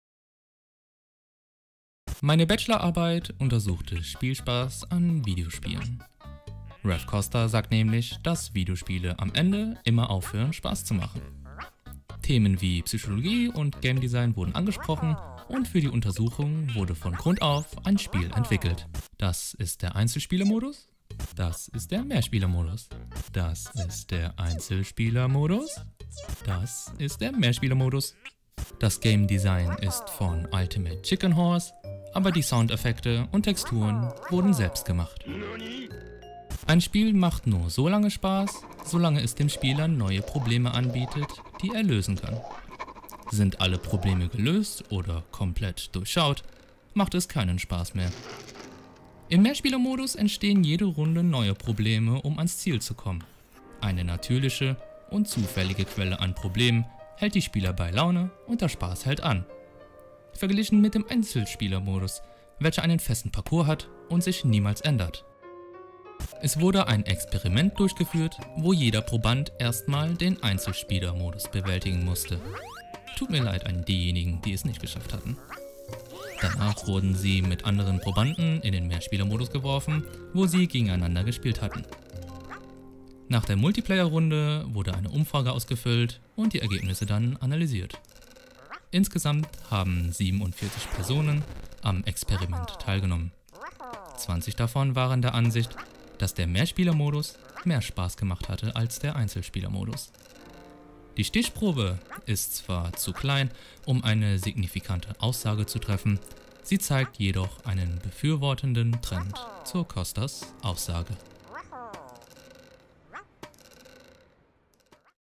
Untersuchung von Spielspass an Mäusefallen
Mithilfe eines selbst entwickelten Videospiels wurde versucht, die Aussage von Raph Koster zu untersuchen. Die Aussage von Koster handelt über Videospiele. Ihr unausweichliches Schicksal ist es nämlich, langweilig zu werden. Für die Umsetzung der Untersuchung wurde das Videospiel „Ultimate Chicken Horse“ als Orientierung für das Game Design genommen, zwei verschiedene Versionen programmiert und anschließend eine Umfrage mit Probanden durchgeführt. Die Ergebnisse liefern einen Trend, der darauf hindeutet, dass die Aussage von Koster
stimmt.